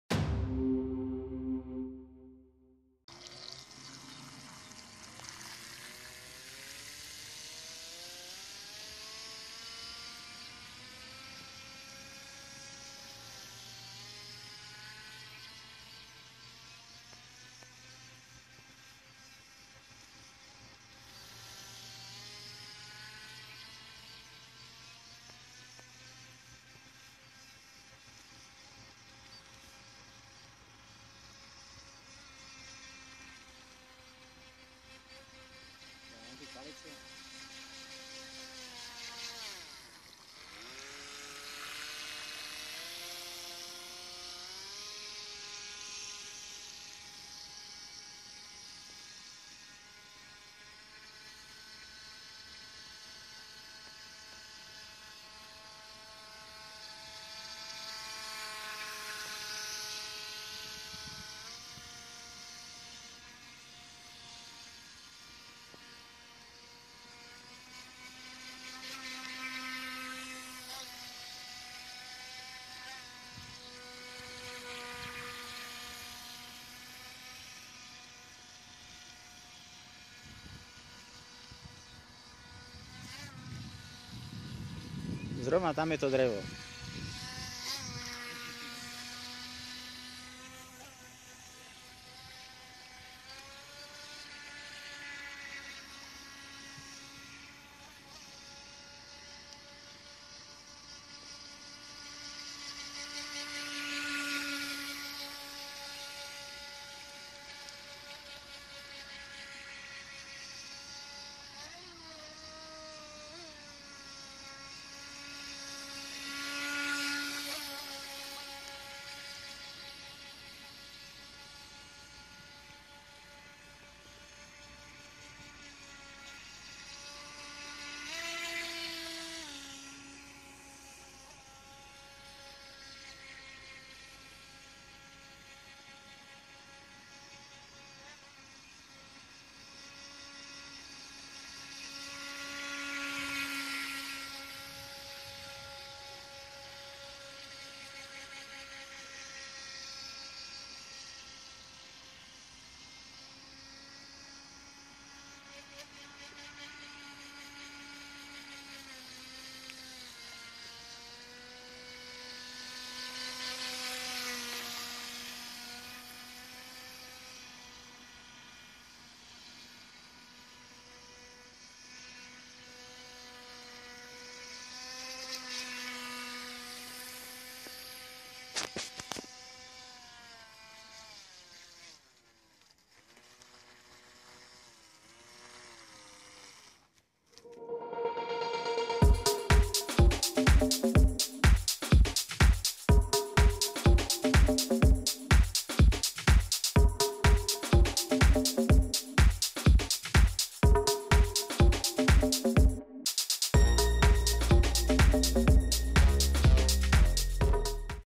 RC Buddy Toys High Speed Boat 350 FT007 cruise on the lake 4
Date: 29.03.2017
Boats:
- Buddy Toys High Speed Boat 350 FT007 RTR from factory
Battery: Li-ion 700mAh